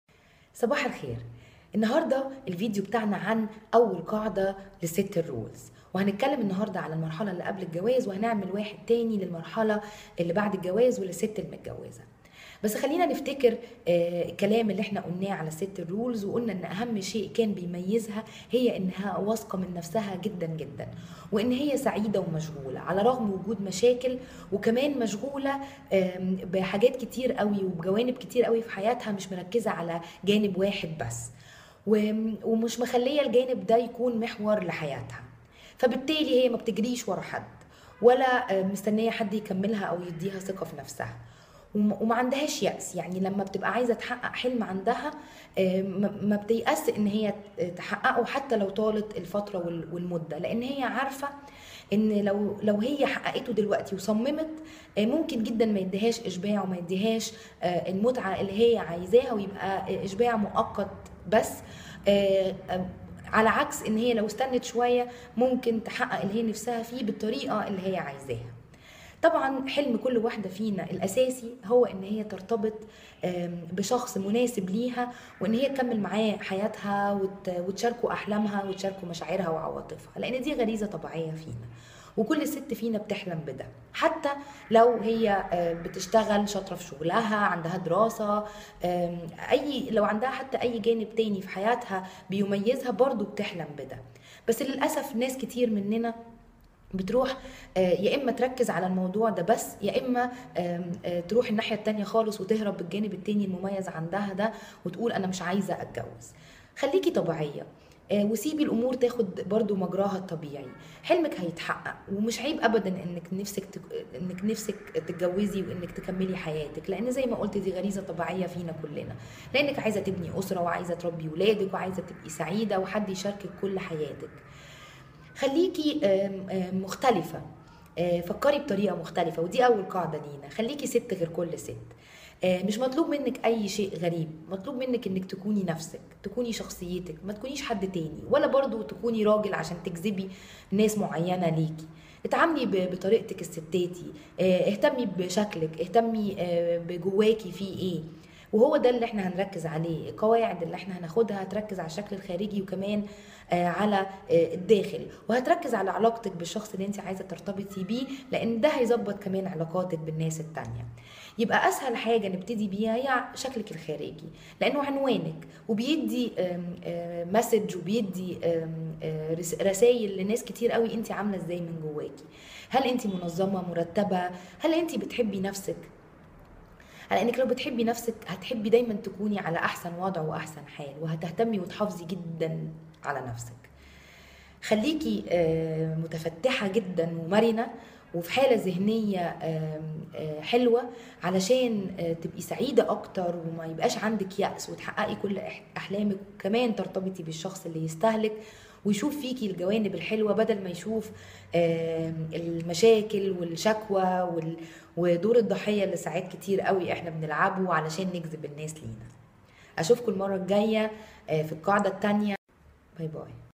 Mariam Osman Relationship & Life Coach Session 4
Relationship & Life Coach
Episode 4
Rules Girl -Rule No. 1 .How to capture the heart of Mr Right .
القاعدة الاولي للست الرولز - الطريقة المجربة لجذب فتي احلامك ...